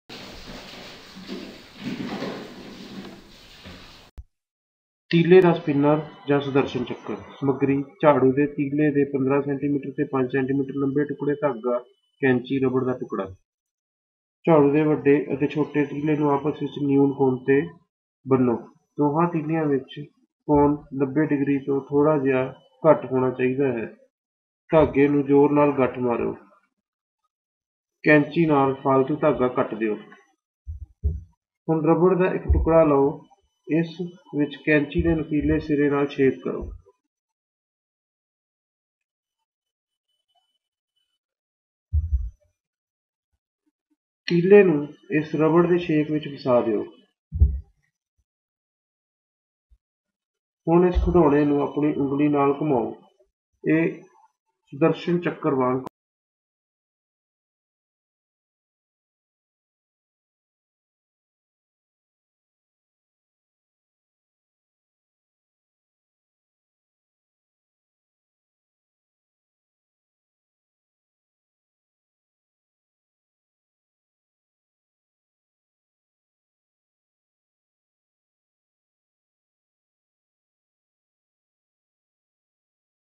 Stick Spinner | Punjabi
This simple demonstrates centrifugal force.
You will need broomsticks, thread and a piece of rubber.
Tie the long and short sticks together at an acute angle.
Trim the excess thread.
Poke a hole in the rubber with the scissors.
Push the long stick in the rubber.
The toy is ready for spinning.
Perch it on your index finger and spin away to glory.